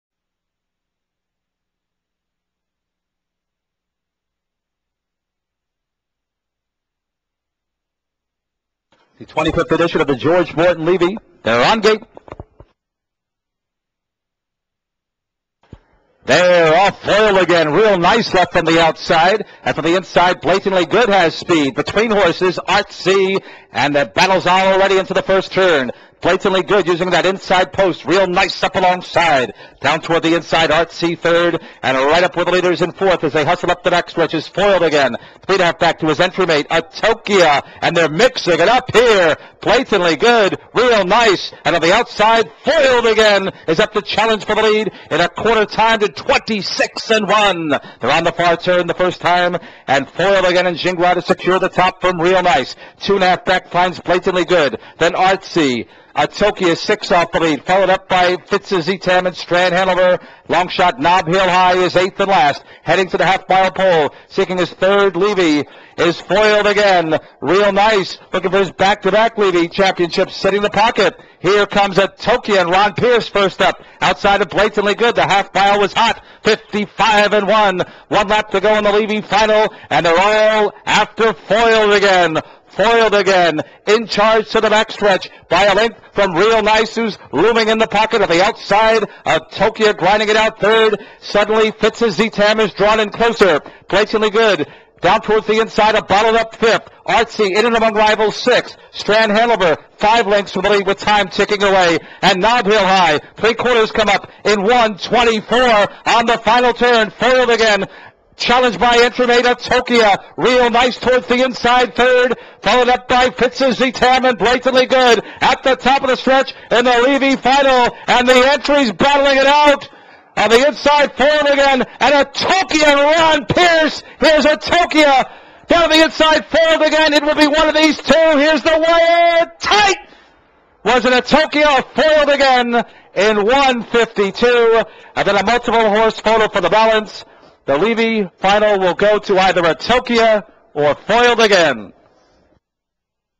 2012 George Morton Levy Memorial Series Final - theharnessedge.com
Atochia wins the 2012 George Morton Levy Memorial Series final on April 28 at Yonkers Raceway.